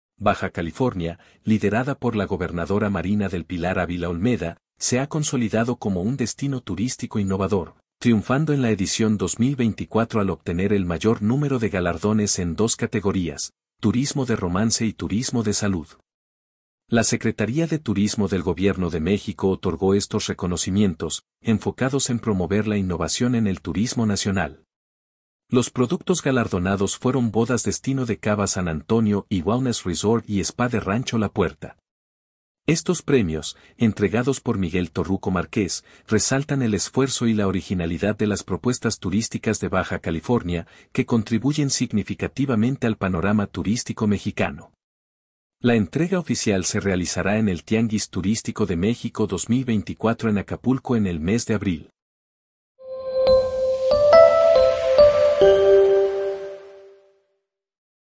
ES BAJA CALIFORNIA UN DESTINO TURÍSTICO INNOVADOR: MARINA DEL PILAR
De entre los 17 estados que compitieron, Baja California se destacó como el gran ganador en la edición 2024, llevándose el mayor número de galardones

MEXICALI.- La Secretaría de Turismo del Gobierno de México resaltó el valor de los productos turísticos bajacalifornianos a través de la entrega del Reconocimiento a la Innovación del Producto Turístico 2024 en dos categorías, indicó la gobernadora de Baja California, Marina del Pilar Avila Olmeda.

La mandataria estatal indicó que las categorías fueron Turismo de Romance y Turismo de Salud. En el primer caso, el producto galardonado fue “Bodas de Destino” de Cava San Antonio, mientras que en la segunda categoría, el reconocimiento fue para “Wellness Resort y Spa” de Rancho La Puerta.

El secretario de Turismo del Gobierno de México, Miguel Torruco Marqués, dio a conocer a los ganadores de este reconocimiento, que tiene como objetivo destacar las iniciativas que contribuyen a la innovación del producto turístico nacional, especialmente aquellas provenientes de pequeñas y medianas empresas turísticas en México.

Marina del Pilar expresó su agradecimiento y destacó la importancia de estos reconocimientos para consolidar la posición de la región como un destino turístico innovador y atractivo, posicionándolo como preferido por visitantes nacionales e internacionales.

"Estos premios son el resultado del esfuerzo constante de nuestra cadena de valor turística por ofrecer propuestas únicas. En Baja California, nos enorgullece ser pioneras y pioneros en innovación, brindando experiencias memorables a nuestros visitantes y consolidando nuestra posición como un destino turístico de clase mundial", expresó.

Mientras que el secretario de Turismo de Baja California, Miguel Aguiñiga Rodríguez, explicó que fue la Dirección General de Innovación del Producto Turístico, el área encargada de otorgar estos premios, que reconocen de esa manera el impacto positivo de las propuestas de Baja Califoria en el panorama turístico nacional.

Asimismo, indicó que la entrega oficial de los premios se realizará durante el Tianguis Turístico de México 2024, el evento más importante de la industria turística en América Latina, que se llevará a cabo entre el 8 y el 12 de abril en la ciudad de Acapulco, Guerrero.